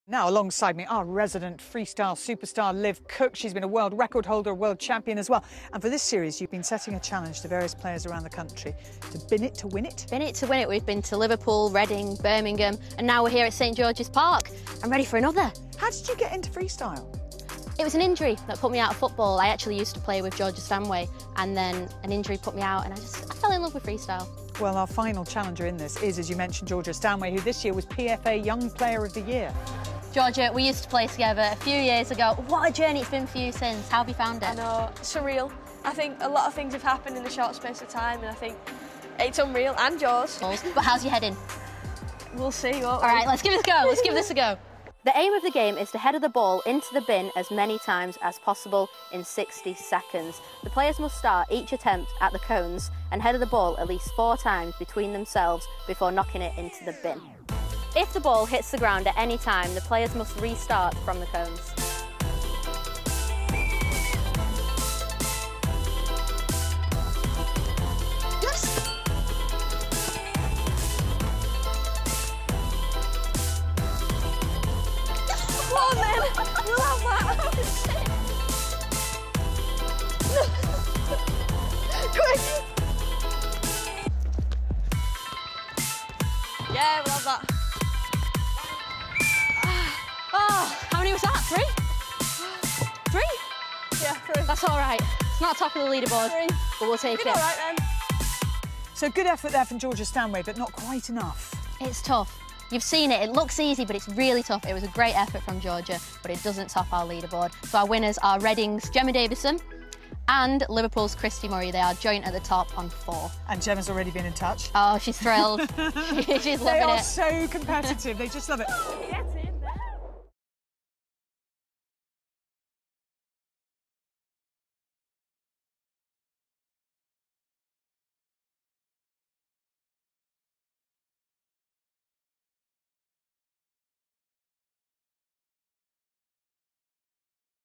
Man City's Georgia Stanway Takes the Header Challenge! | Women's Football World with Coca-Cola
Liv Cooke is joined by Man City's Georgia Stanway to take on the 'Bin It To Win It' header challenge!